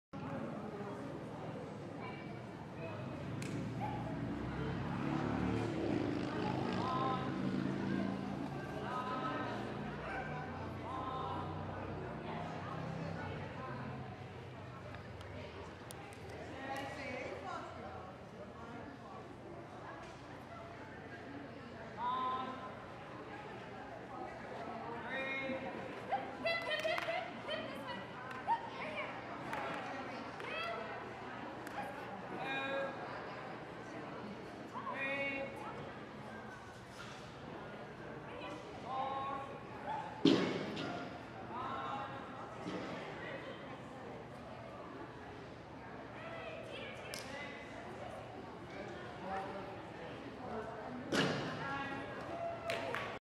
Team Little Bits CPE Agility Nationals 2013 - Pip Snooker C
Snooker, this was not an easy Snooker (are any?) so I was really happy to come out with a Q!  46.58", 44 points, Q.
Video by Agile Images pip 06092013 snooker 1280